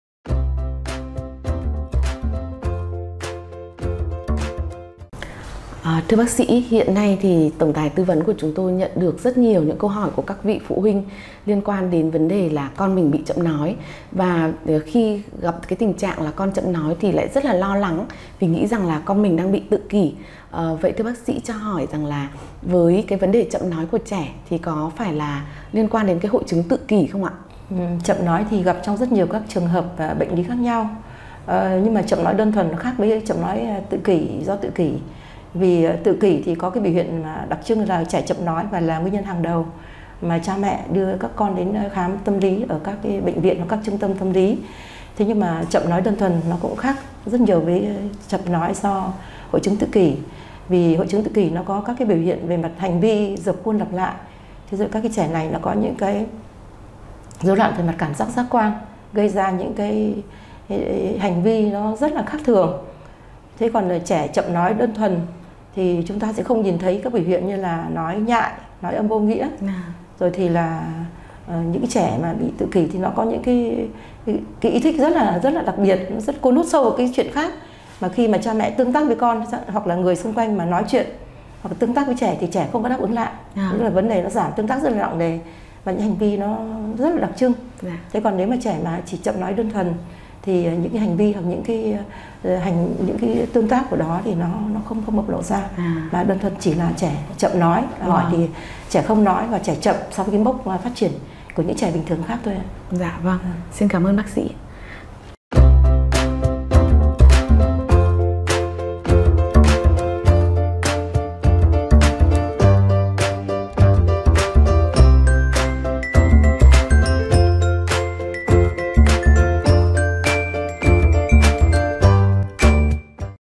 Trẻ chậm nói có phải tự kỷ không
Chậm nói gặp rất nhiều trong các bệnh lý khác nhau. Chậm_nói_đơn thuần khác với chậm nói do tự kỷ.
Trẻ_chậm_nói_do_tự_kỷ thường có các biểu hiện bất thường về hành vi dập khuôn lặp lại, rối loạn về cảm giác, giác quan. Khi nói chuyện với trẻ, bé không đáp ứng lại.
Chậm_nói_đơn-thuần là trẻ chậm nói so với mốc phát triển thông thường và không có những biểu hiện bất thường về hành vi.